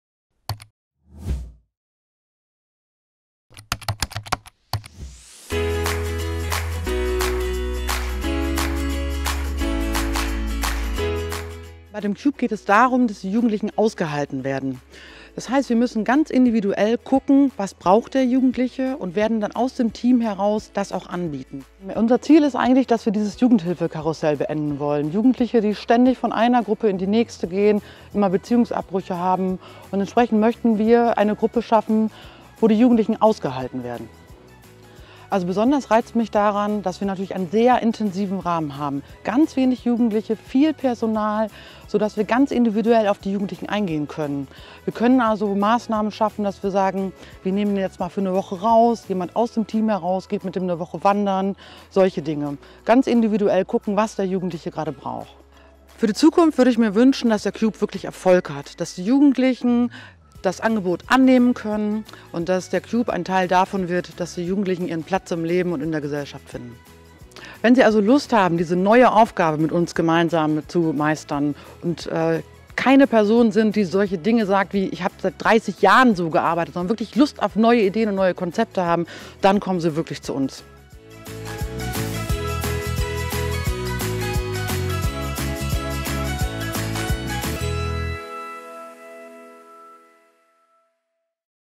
Graf Recke Karriere – Interview mit Anne Hans: Projekt "Cube"
Der Cube in Düsseldorf-Wittlaer bietet traumatisierten Kindern und Jugendlichen einen sicheren Sozialraum, wo sich das Ankommen lohnt und wo sie und ihr Bleiben gewünscht sind. Nach einer Odyssee, die häufig durch verschiedene Einrichtungen geführt hat, finden die Bewohner im Cube einen Ort der Zuflucht. Hier werden sie angenommen und respektiert, wie sie sind. Hier hat jede und jeder mindestens eine zweite Chance verdient.